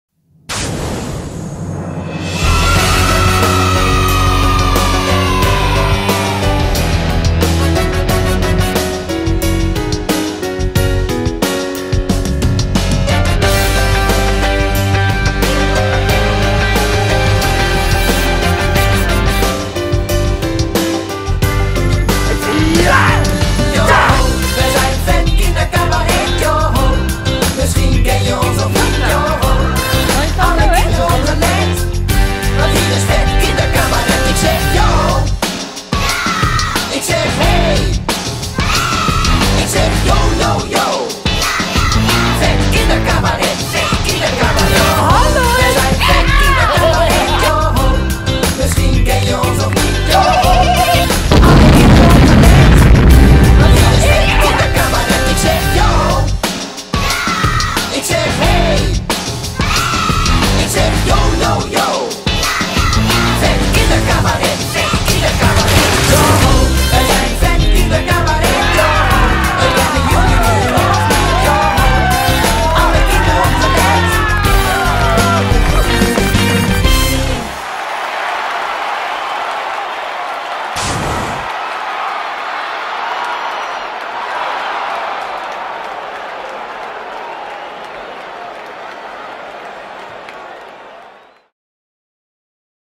VET Kindercabaret official trailer: Vetter dan VET
e waren al VET, maar nu zijn ze VETTER dan VET!
VET kindercabaret komt alweer met de vierde show en blijft verrassen met hun zeer energieke en muzikale voorstellingen.

Het gaat in VET4 over loze beloftes van je ouders, over vet coole beugelbekkies, over opschieten in de ochtend en het nieuwe jongetje in de groep. VET neemt je mee naar de kinderboerderij, maar of de geitjes dat ook leuk vinden...

Met herkenbare sketches waar jij en je ouders heel hard om kunnen lachen en vetleuke liedjes om heel vaak mee te zingen!
VET Kindercabaret, leuk voor het hele gezin (6+)!